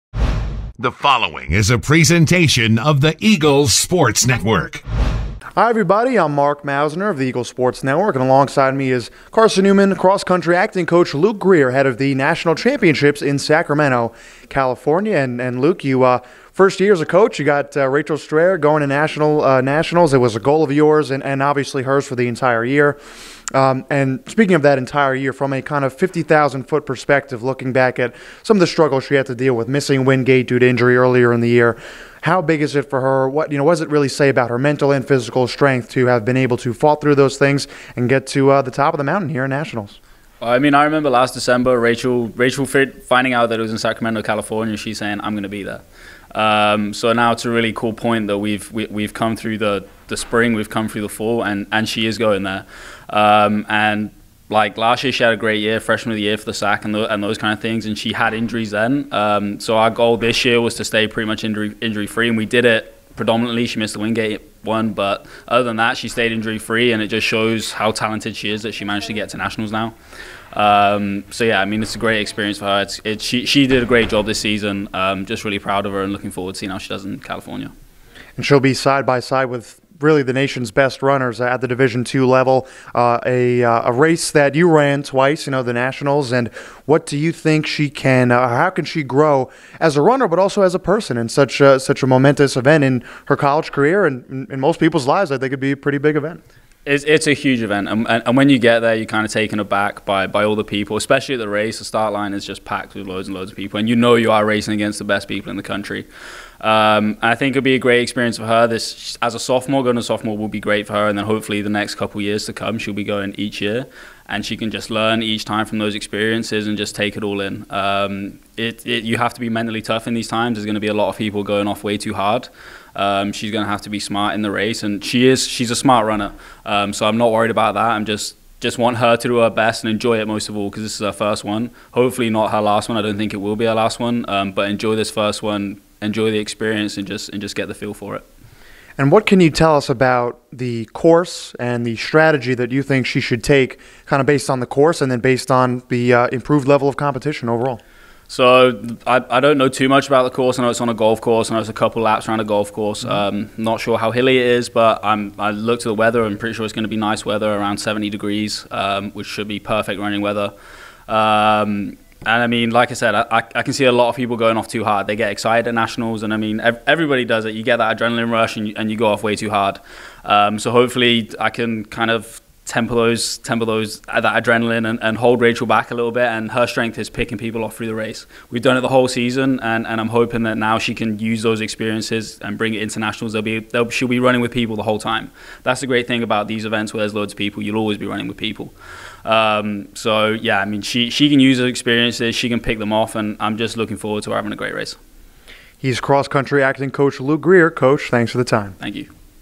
Carson-Newman Cross Country: Luke Greer previews NCAA National Championships 11-21-19
Carson-Newman acting cross country coach Luke Greer chats about sophomore Rachel Strayer's trip to Sacramento, California for the 2019 NCAA DII Cross Country National Championship